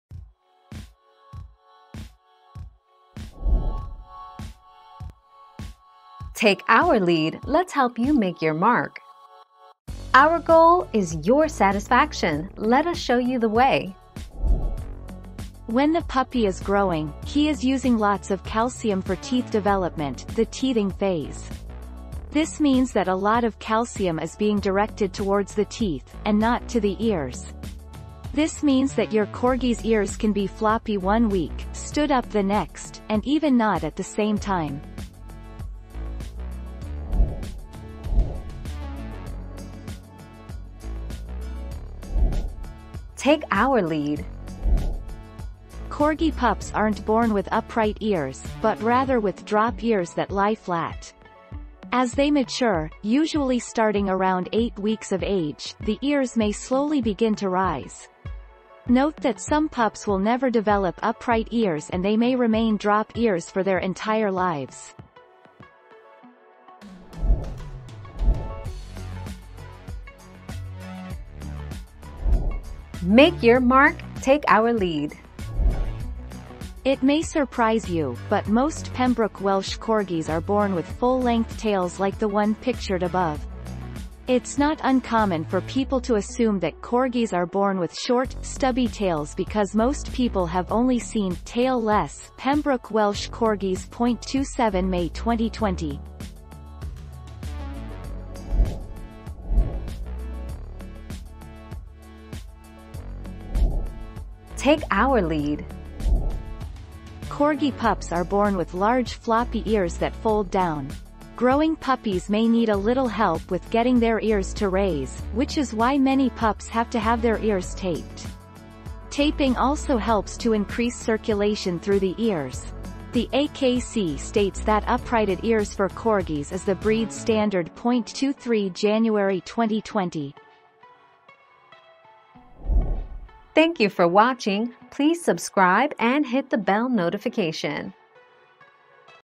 Why do corgi ears stand up?
00:00 - Why do corgi ears stand up?
00:39 - Do corgi ears stay floppy?
01:08 - Are corgis born with tails?
01:40 - Do corgis get their ears cropped?

Laura S. Harris (2021, February 24.) Why do corgi ears stand up?
    AskAbout.video/articles/Why-do-corgi-ears-stand-up-221656